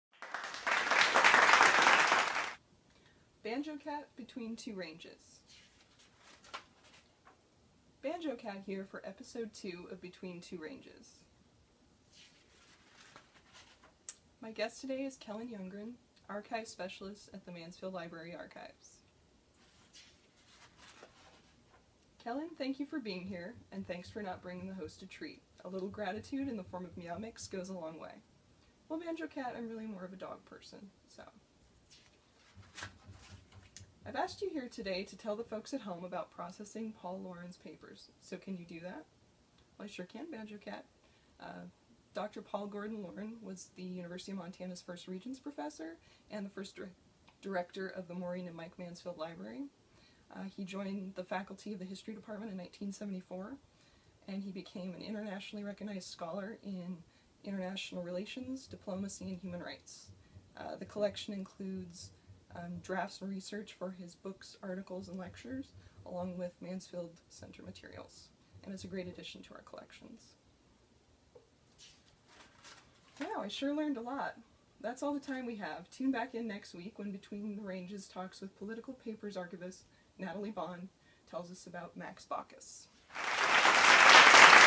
Banjo Cat: Between Two Ranges Episode 2
Mansfield Library Archives & Special Collections Mascot Banjo Cat interview show titled "Between Two Ranges" interviews archives specialist Kellyn Younggren.